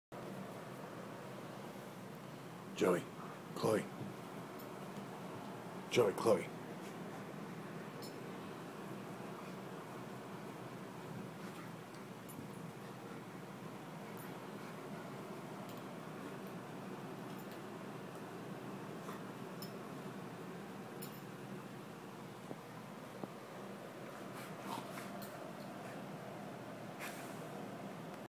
Zombie Dog Fight
My dogs are possessed. I can see it in their eyes.